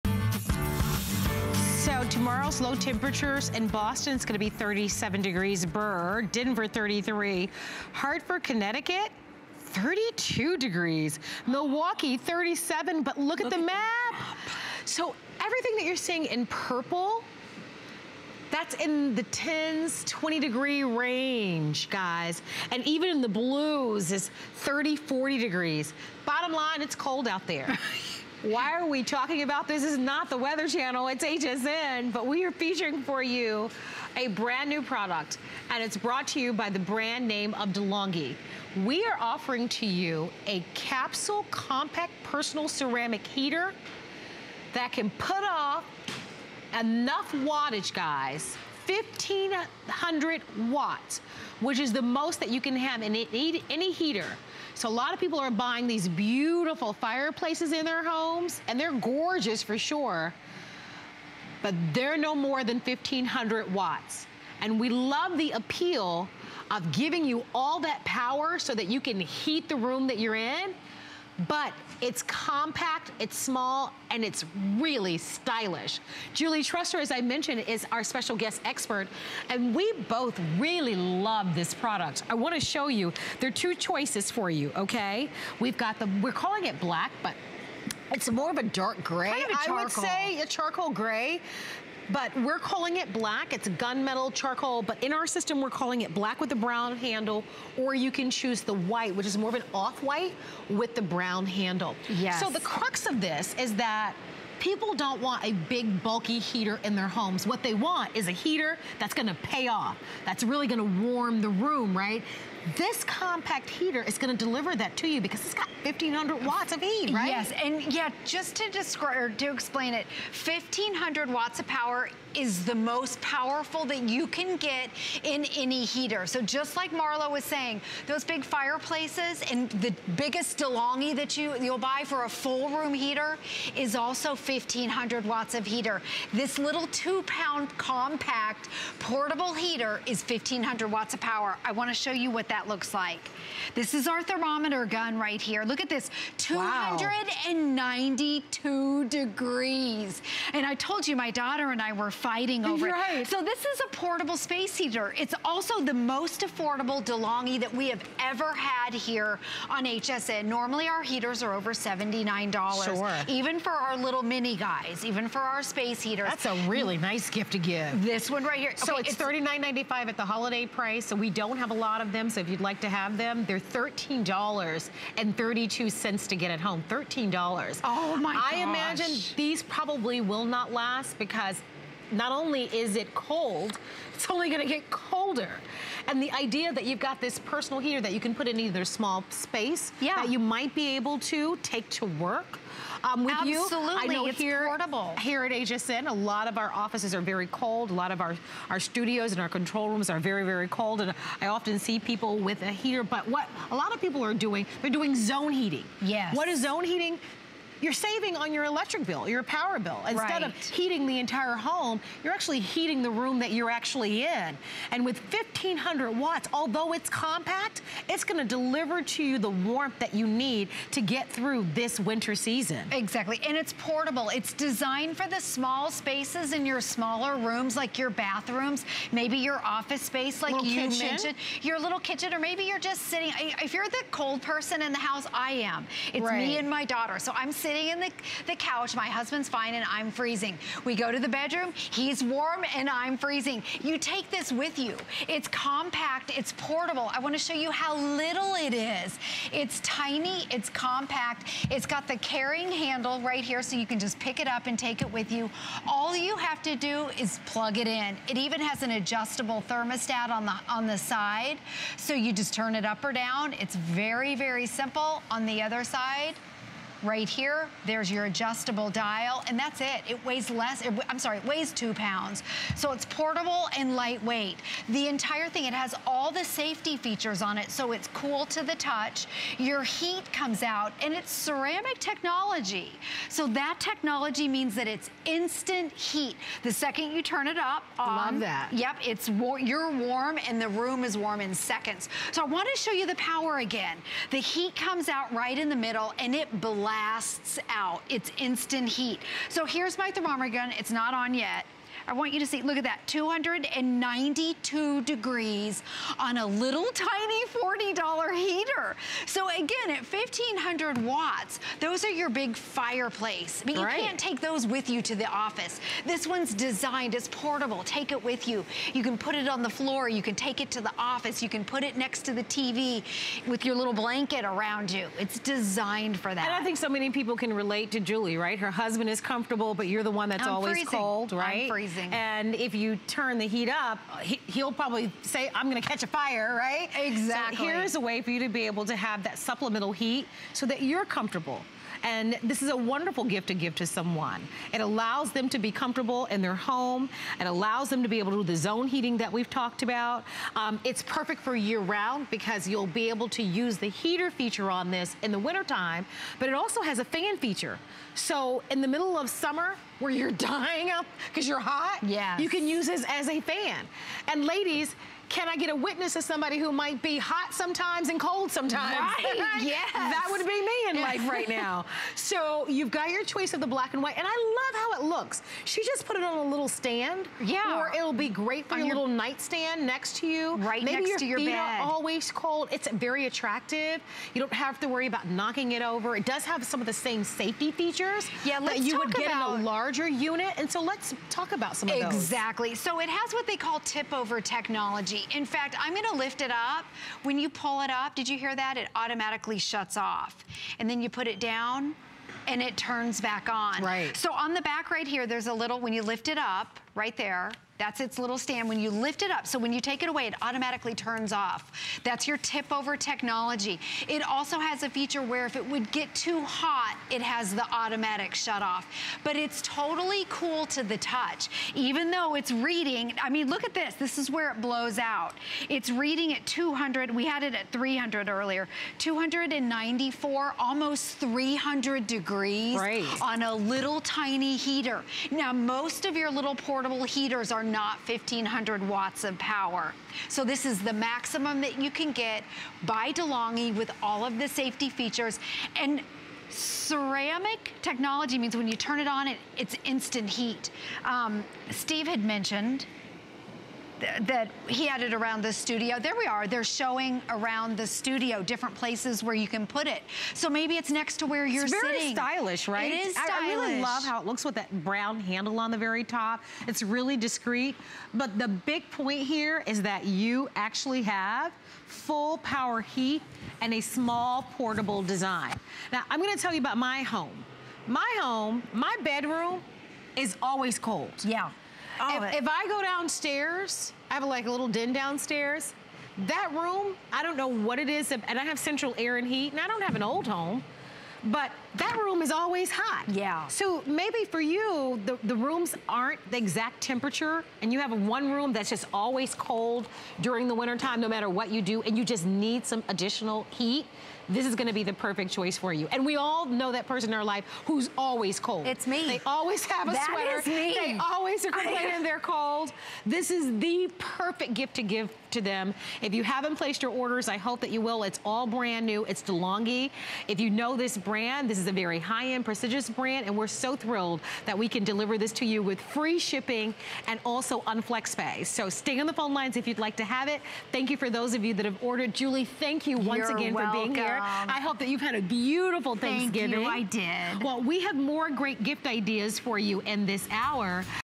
De'Longhi 1500W Capsule Compact Personal Ceramic Heater
De'Longhi 1500Watt Capsule Compact Personal Ceramic Heater
Stay cozy and warm at home or at your work desk with this compact, portable heater. The perfect size for personal heating, it offers...